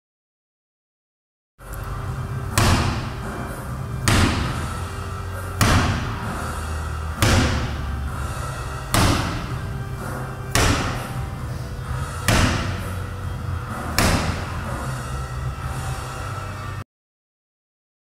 Wolf Brigade- 1-arm slam ball (20lb.)
More a dexterity drill and "Odd Lift" than a curriculum priority, 1-arm slam ball is an interesting experiment in unilateral coordination and timing (Don't punch the floor).